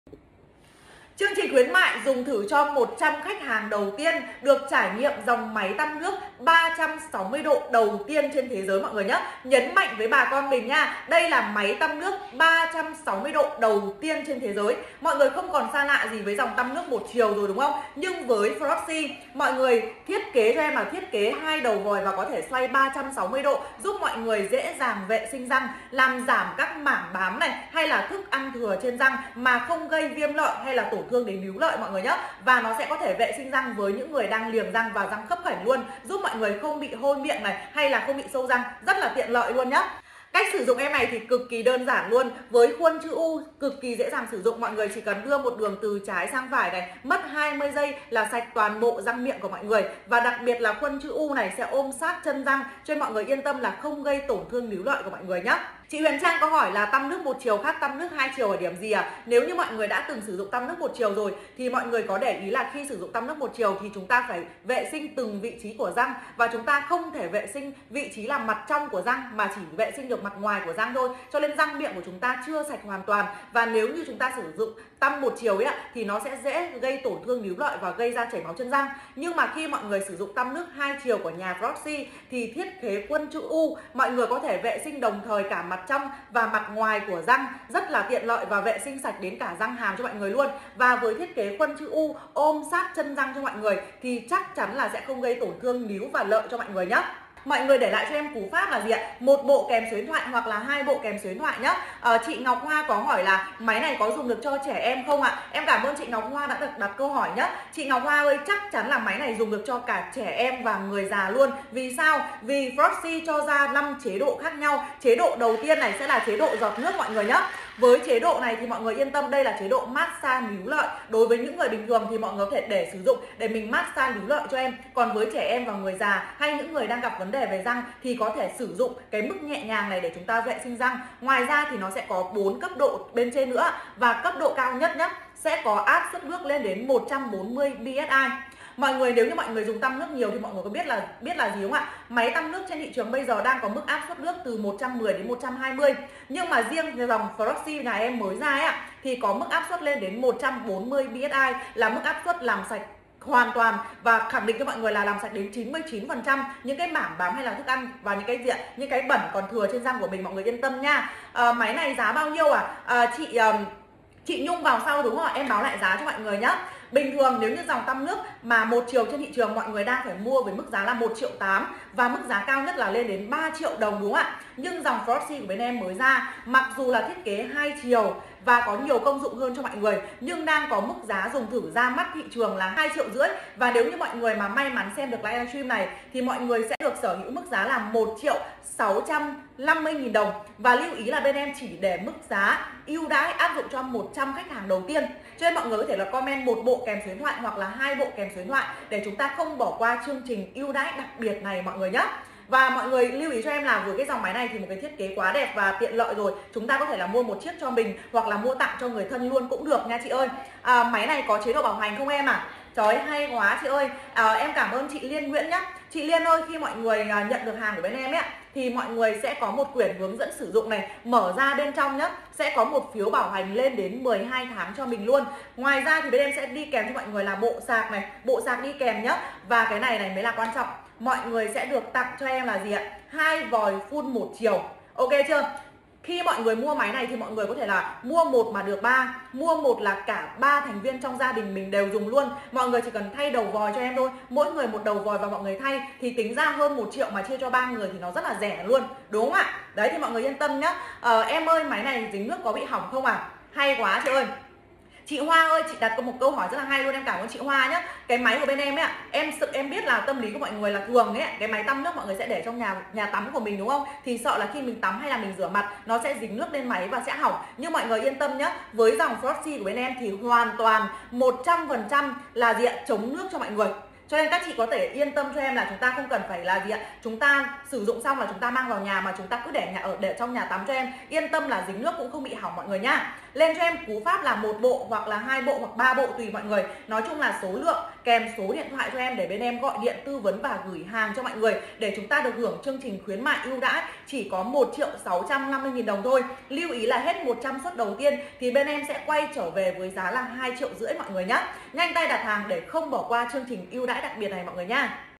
Reivew chân thực về máy tăm nước 2 chiều hot hit
👉 6 Điểm nổi bật của Tăm nước 2 chiều Flossie:
- Vòi phun 2 chiều làm sạch cả mặt trong và mặt ngoài hàm răng
- An toàn cho nướu & chân răng nhờ cơ chế cố định tia nước
- Áp lực tối đa 140PSI đầy mạnh mẽ, có khả năng đánh bật thức ăn thừa & mảng bám ở những nơi khó tiếp cận nhất
- Linh hoạt với 5 mức điều chỉnh lực nước, phù hợp với đa số nhu cầu sử dụng.
- Bộ vòi phun đa dạng, phù hợp với nhiều loại răng
- Thiết kế gọn nhẹ, có túi đựng để đem đến cơ quan, mang đi du lịch